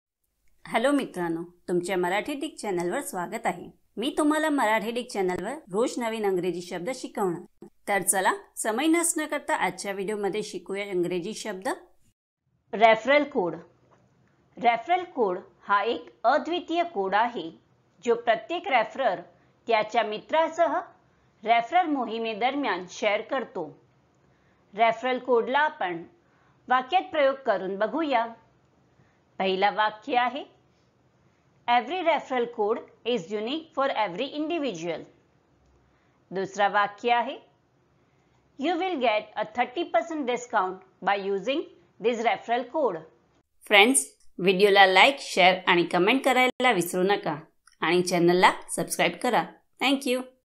Referral Code Meaning in Marathi | Referral Code म्हणजे काय | Referral Code in Marathi Dictionary
या व्हिडिओमध्ये तुम्हाला मराठीत Referral Code चा अर्थ समजेल आणि Referral Code चा उच्चारही शिकाल. याचा अर्थ तुम्ही Referral Code चा अर्थ जाणून घ्याल तसेच Referral Code कसे उच्चारता येईल, Referral Code म्हणण्याचा योग्य मार्ग कोणता आहे.

तर Referral Code शब्द फक्त एका मिनिटात शिका.

उदाहरणांसह Referral Code समजून घेऊ.

Referral Codemeanininginmarathi
Referral Codemeaning
Referral Codeinmarathi